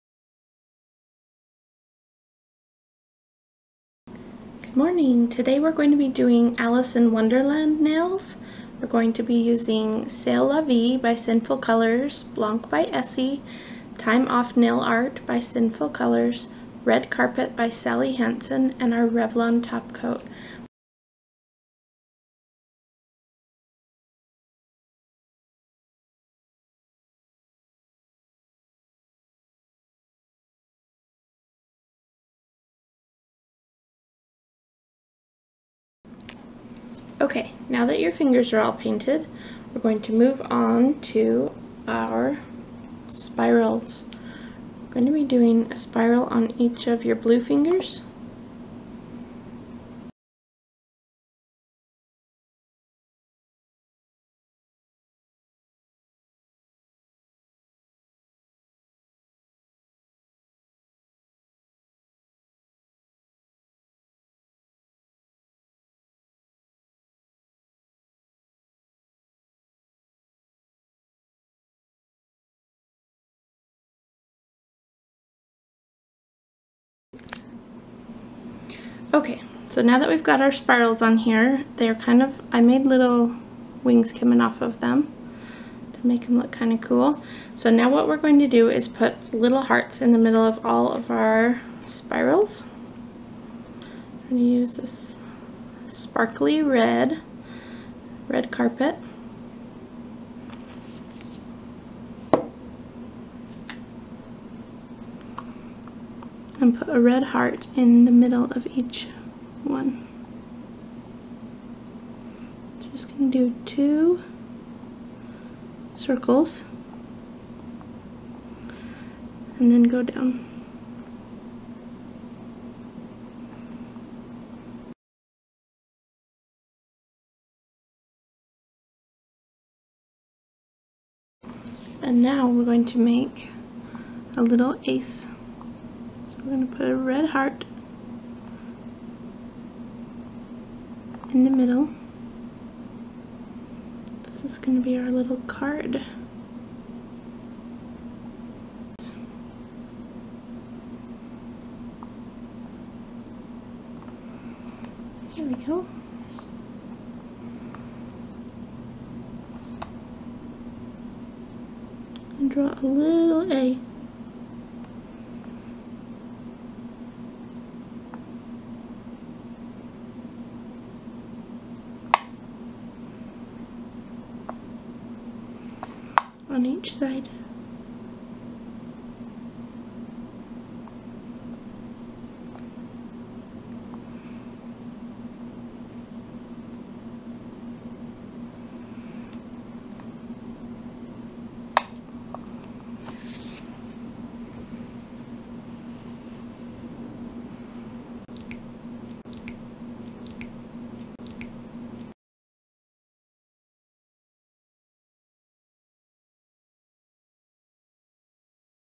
Alice in Wonderland Nails
Hello Everyone! I'm Alison! Today I'm doing Alice in Wonderland Nails! My daughter is going to be Alice for Halloween. I painted her toes blue this morning with a little rhinestone on them. They turned out so cute! This video is short because I did 3 videos today. The other 2 should be uploaded soon, the Candy Corn nails and Silver Spider Web nails. Check them out too!

As a little girl I was called Alison Wonderland all the time at school. I would always explain that her name is ALICE in Wonderland, not Alison... :) I thought the cartoon Alice was quite silly back then but when the new Tim Burton film came out I saw Alice as extremely brave. That movie is incredible!

Here's What I used:
Sail La Vie by Sinful Colors
Blanc by Essie
Time Off Nail Art by Sinful Colors
Red Carpet by Sally Hansen
and Revlon Top Coat

Remember you are LOVED!

~Me